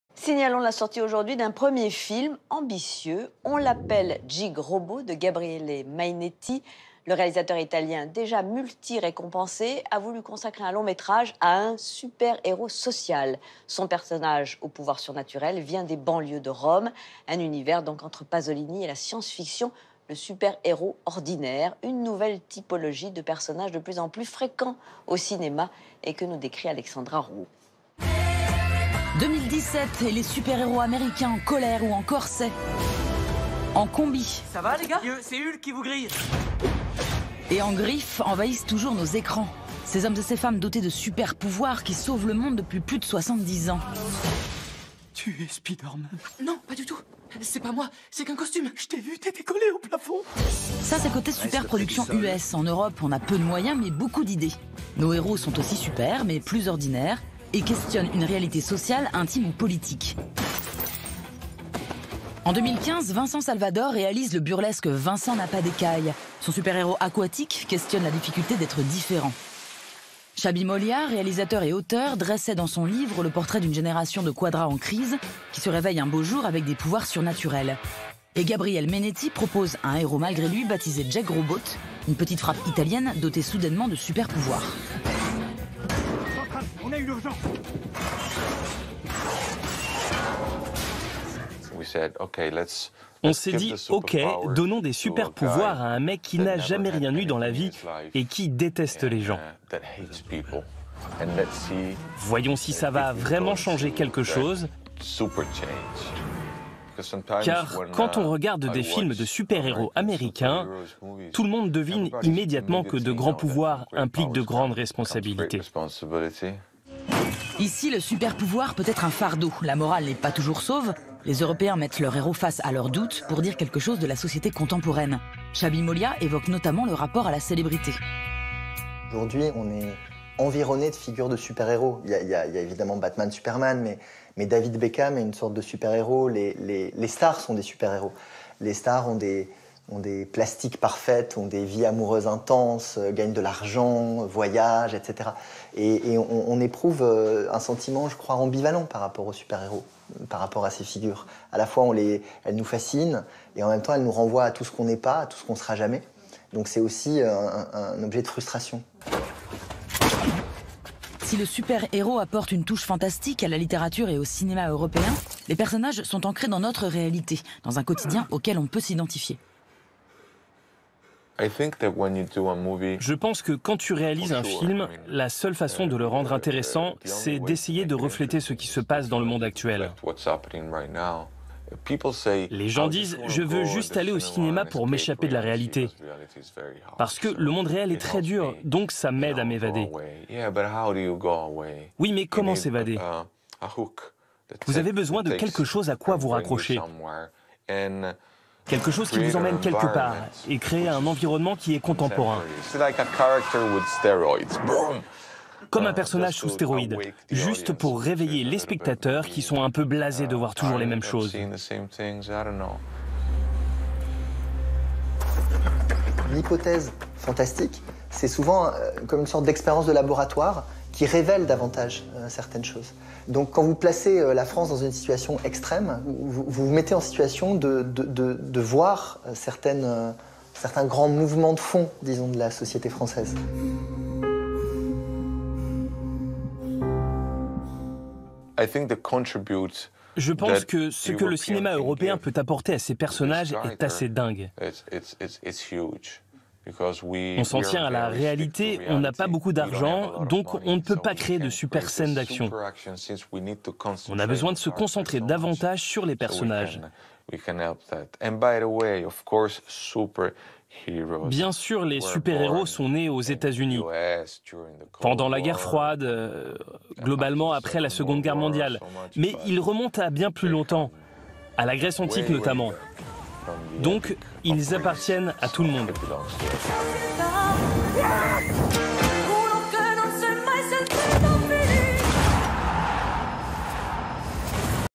Les héros marchent à l'ordinaire
Loin des blockbusters américains, le cinéma européen emploie souvent le fantastique pour mettre à l’honneur ces héros « ordinaires ». De « Vincent n’a pas d’écailles » à « On l'appelle Jeeg Robot » en passant par Les Premiers de Xabi Molia, Entrée Libre revient sur le phénomène.